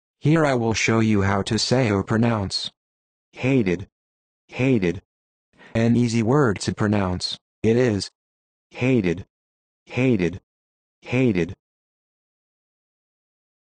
How to Say or Pronounce Hated
This video shows you how to say or pronounce Hated.

How accurate does it say Hated? How would you say Hated?